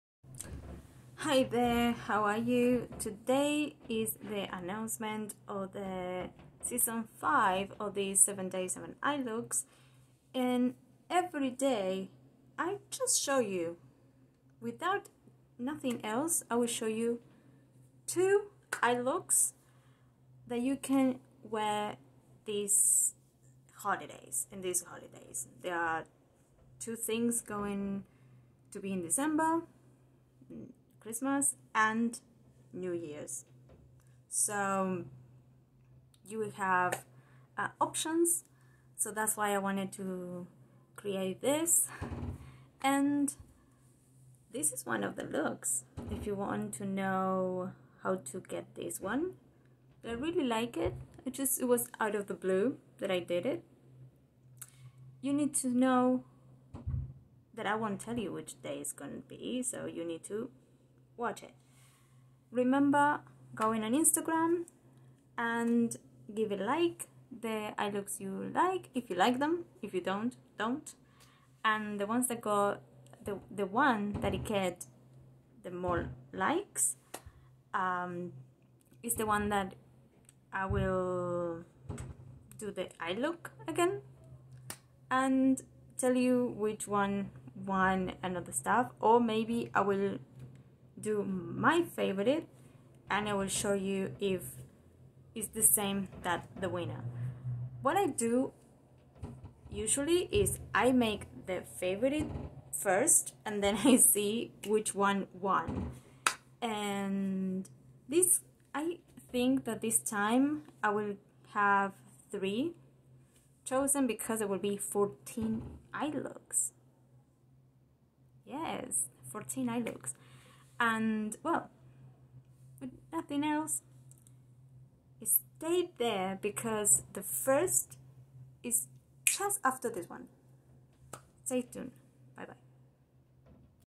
7 days 7 eyelooks - S5 - Announcement
This is the video where I explain the looks and how they'll be posted. 2 eye looks each day. 14 total so you have some to choose from as there will be Christmas and New Year.
On my IG I will post each day the looks and the one with more likes, wins.
Each video have the detail and the real time application of each look.

Social media (Twitter, Snapchat and Instagram) @dbmparanoid
Camera used: Moto G8 Plus. Front. Full HD.

If you enjoyed this video, please give it a Thumb Up.